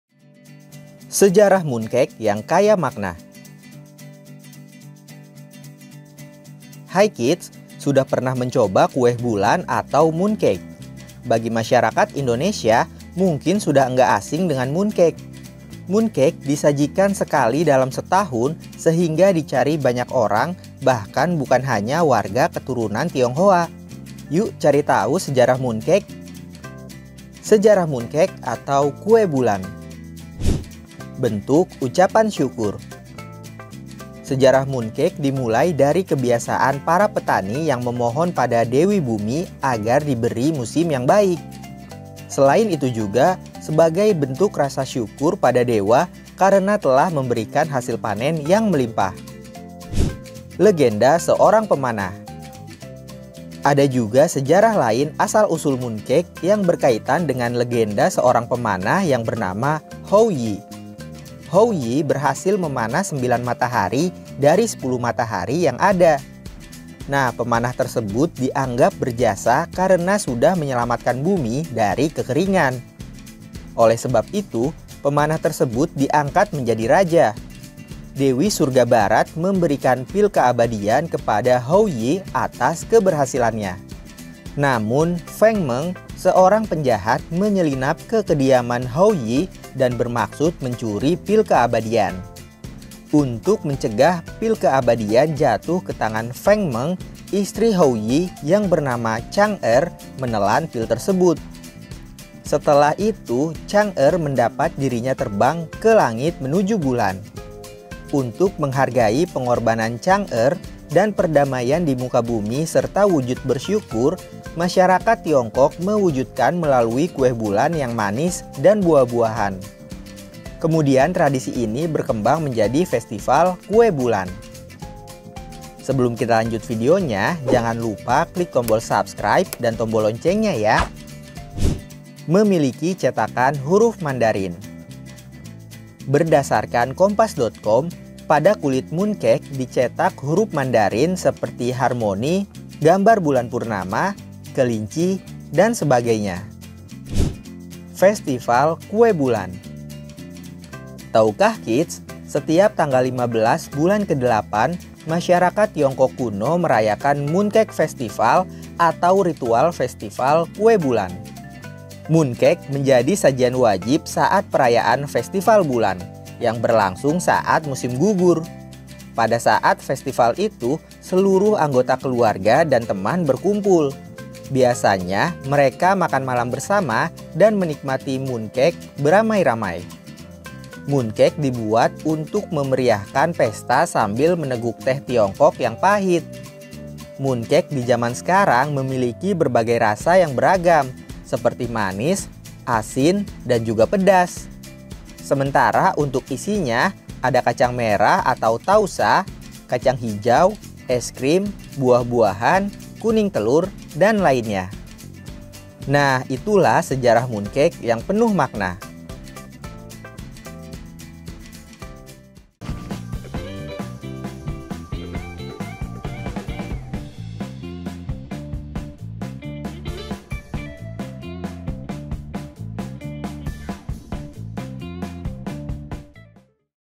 Fakta Menarik Kue Bulan - Sejarah Mooncake yang Kaya Makna
Mooncake disajikan sekali dalam setahun sehingga dicari banyak orang bahkan bukan hanya warga keturunan Tionghoa. Yuk cari tahu sejarah mooncake!

GRIDSKIDS ARE ALL ABOUT FUN!

Produser: David Togatorop
Editor: Sigit Purnomo, Iveta Rahmalia
Video Editor & Videografer: Inggar Rivaldi
Graphic Designer: Aisya Gilda